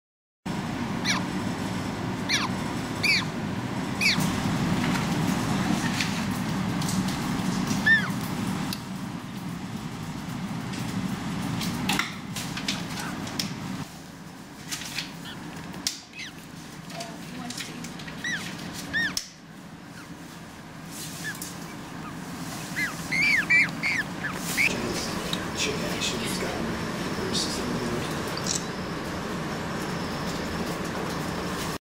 Penguin chick hatched at Adventure Aquarium
- Say hello to the newest member of the Adventure Aquarium family! Watch as our newest penguin chick hatches from its shell and joins its sibling.